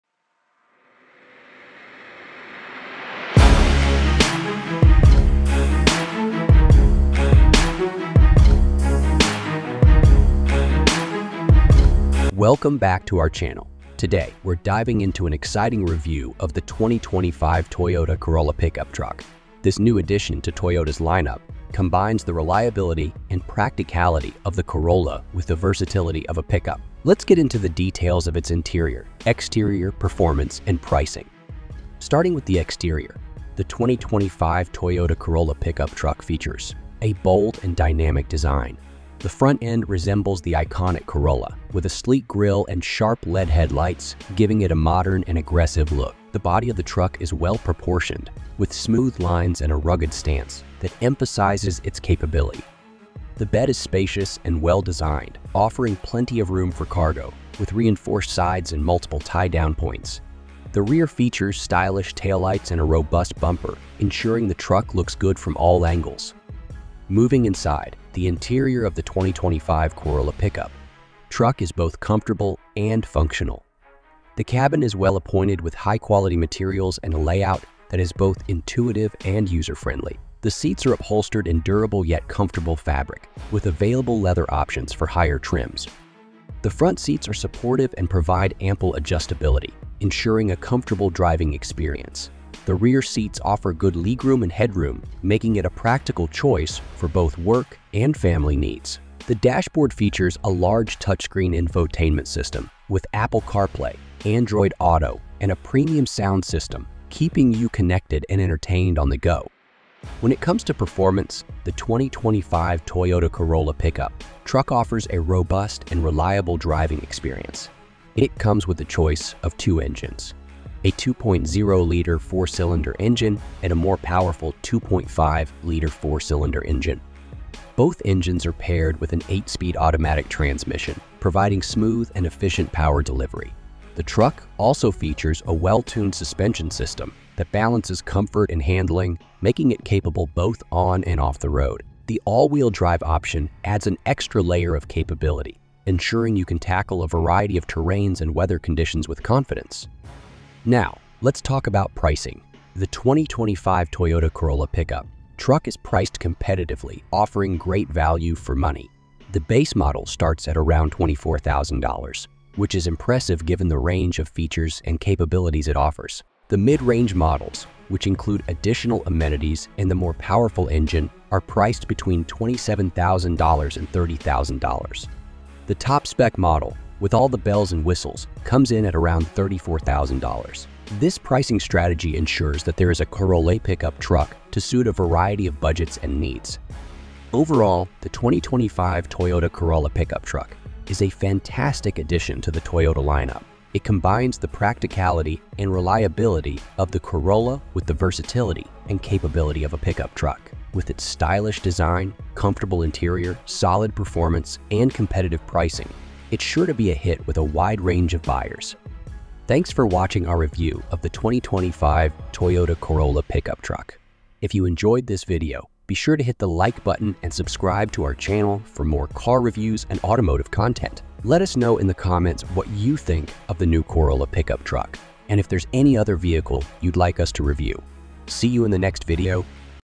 2025 Toyota Corolla Pickup Truck – The Future of Compact Trucks”
2025 Toyota Corolla Pickup Truck – The Future of Compact Trucks”

The 2025 Toyota Corolla Pickup Truck is here, redefining what a compact truck can be. Combining Toyota's renowned reliability with a sleek, modern design, this pickup offers exceptional performance and versatility. Whether you're hauling gear for work or heading out on an adventure, the 2025 Corolla Pickup delivers with style and efficiency. Join us as we explore its features, performance, and why it's set to become a favorite among truck enthusiasts.